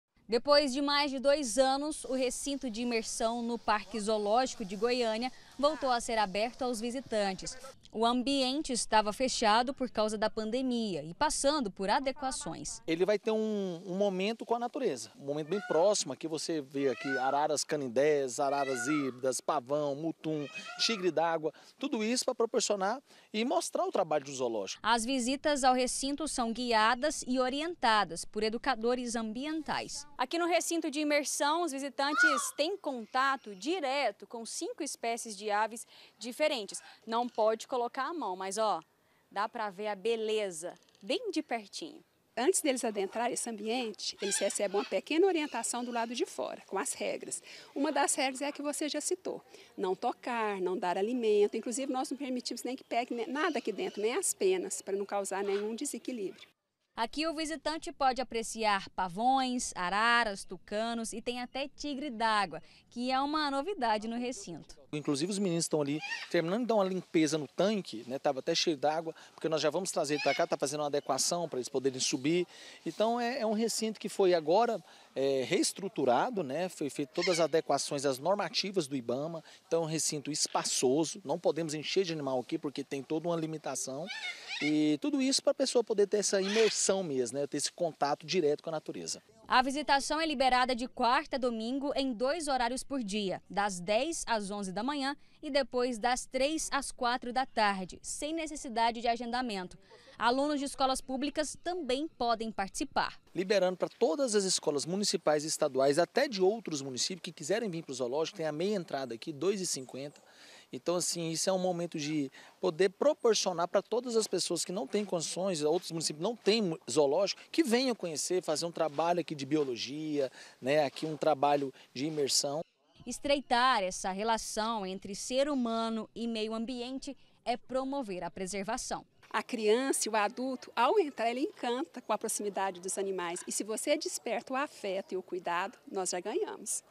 EDUCAÇÃO AMBIENTAL: ZOOLÓGICO DE GOIÂNIA PROMOVE VISITAS GUIADAS
O ZOOLÓGICO DE GOIÂNIA RETOMOU AS VISITAS GUIADAS NO RECINTO DE IMERSÃO, AQUELE EM QUE A PESSOA PODE ENTRAR E INTERAGIR COM OS BICHOS.